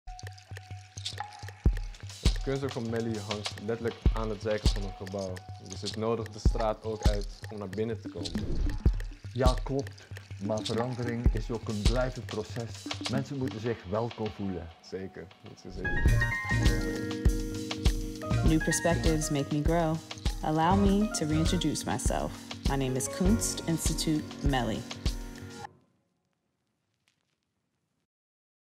Kunstinstituut Melly — New Perspectives Make Me Grow
Welcome to Kunstinstituut Melly

We launch our new name with an online program on Wednesday, 27 January 2021 at 8 pm. From then onwards, the institution formerly known as Witte de With Center for Contemporary Art will be called Kunstinstituut Melly.

One in a series of video clips featuring dialogues with longstanding and new collaborators: Ed de Meyer (community worker, Cool District; since 1992, co-organizer of art projects with us) with Mohamed Chajid (participant of the first edition of the Work/Learn Project, in 2018); dancer Chanel Vyent (public program participant 2020) with fashion designer Odair Pereira (tutor Work/Learn Project, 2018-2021), audience members, Jeroen Koolhaas with his daughter Lua Koolhaas; Yootha Wong-Loi-Sing with Jacques Wong-Loi-Sing (both family members of Cye Wong-Loi-Sing, who is part of the name change initiative workgroup since 2020 and director of this media campaign).